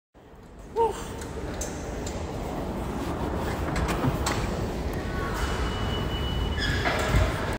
Don’t touch Subway turnstile near Brooklyn Nets Arena ! Sewer rats 🐀
Brooklyn Nets subway turnstiles is hella nasty! Sewer rats and everything!
Barclays Center